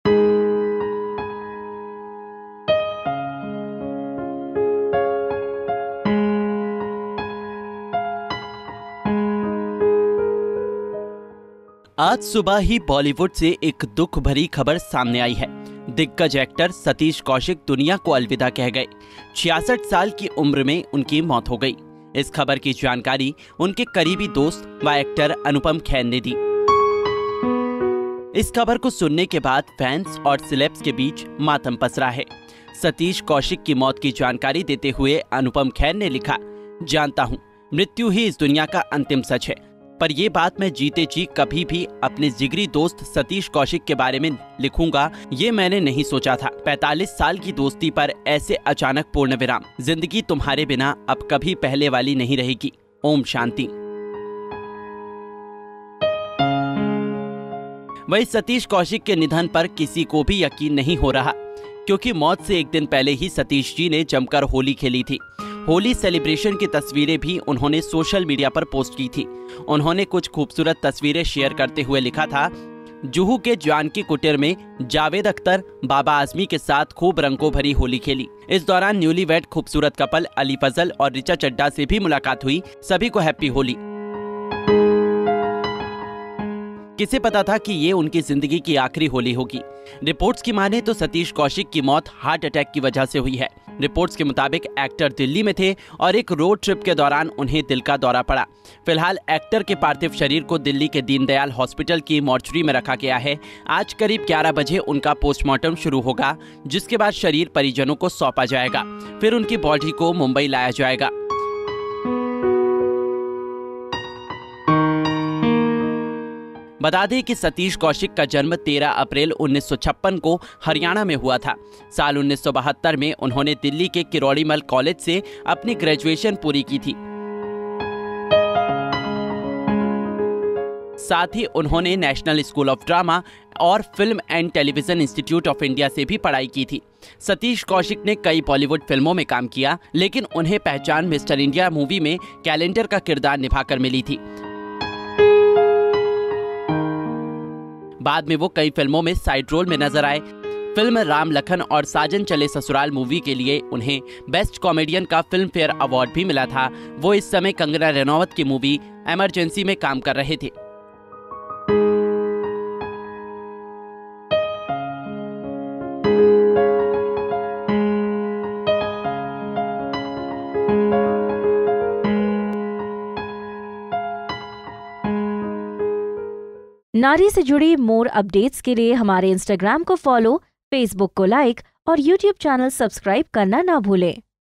एक दिन पहले खेली होली, अगले दिन ही कहा दुनिया को अलविदा, नहीं रहे दिग्गज एक्टर सतीश कौशिक
एक दिन पहले खेली होली, अगले दिन ही कहा दुनिया को अलविदा, नहीं रहे दिग्गज एक्टर सतीश कौशिक, एक्टर की मौत से टूटे स्टार्स, करीबी दोस्त ने कहा-जिंदगी तुम्हारे बिना अब कभी पहले वाली नहीं रहेगी!.